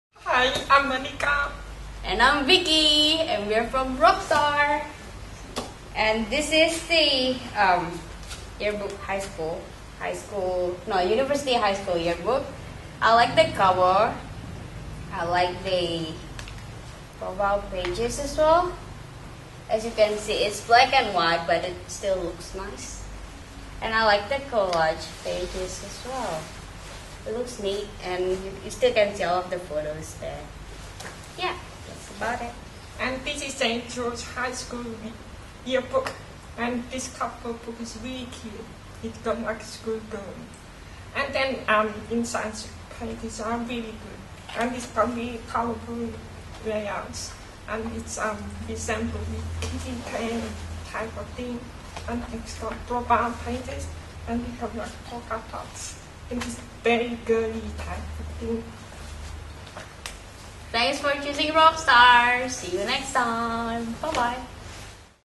University High School/St George Girls High School 2010 Yearbooks
Two for the price of one!! - Congrats to University High & St George Girls' for producing brilliant 2010 Yearbooks! Congrats to those involved - it looks amazing!